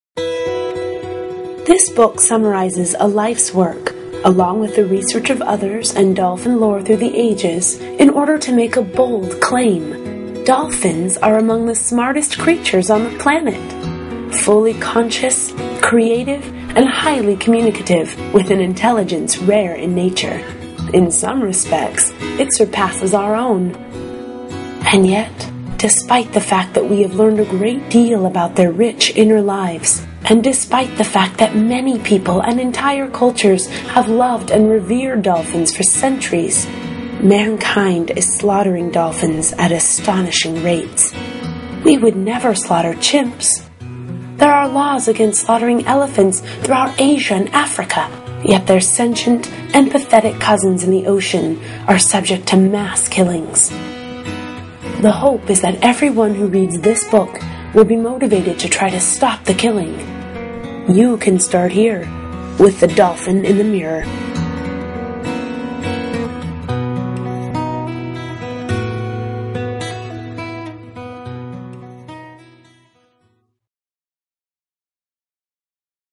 The Dolphin in the Mirror trailer
Author: Diana  Reiss

Title:
The Dolphin in the Mirror

Description:
This book celebrates dolphins' creativity, playfulness, heightened level of self-awareness, and their exceptional ability to communicate with humans, with the author's many breakthroughs in the field of animal intelligence. She has also fought to abolish a barbaric annual Japanese ritual, hunting dolphins for their meat